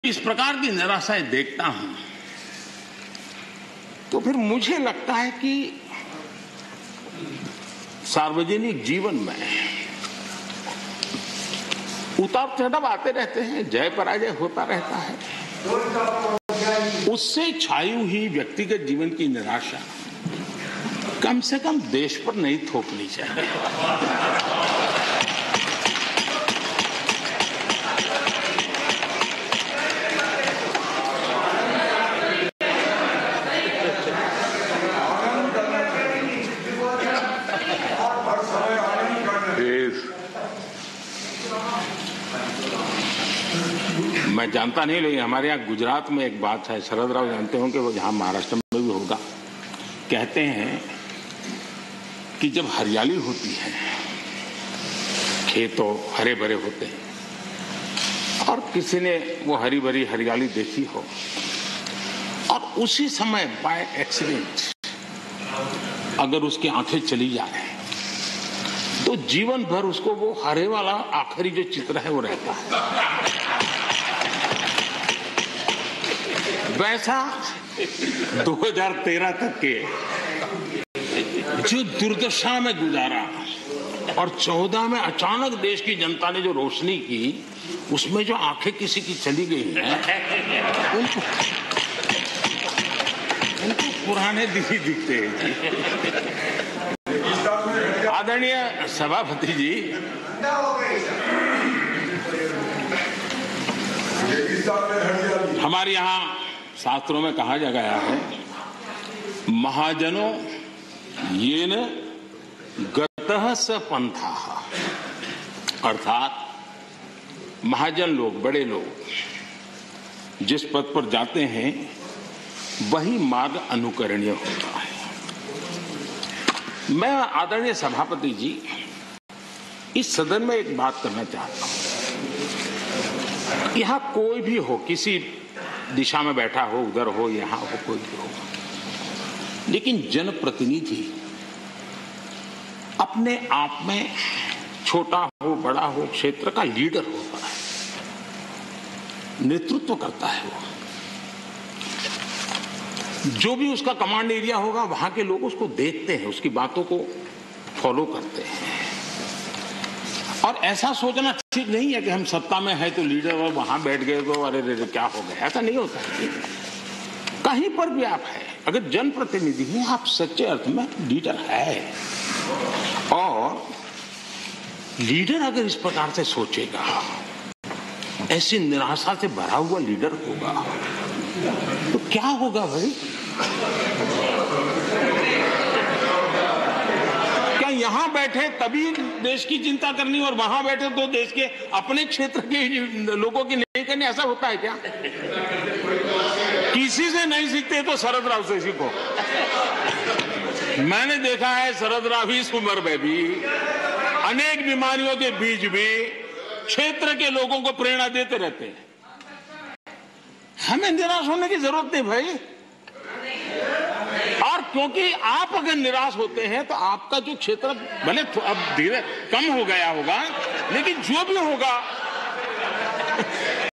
'कुछ साथियों ने पेश की भारत की निराशाजनक तस्वीर' - राष्ट्रपति के अभिभाषण पर PM Modi | Hindi News
राष्ट्रपति के अभिभाषण पर Rajya Sabha में भाषण देते हुए PM Modi ने कहा कि इस सदन में कुछ साथियों ने भारत की निराशाजनक तस्वीर पेश की और ऐसा लग रहा था कि उन्हें इसे पेश करने में आनंद भी आ रहा था। मुझे लगता है कि सार्वजनिक जीवन में उतार-चढ़ाव आते रहते हैं और जय-पराजय होती रहती है, उससे छाई हुई व्यक्तिगत जीवन की निराशा कम से कम देश पर नहीं थोपनी चाहिए।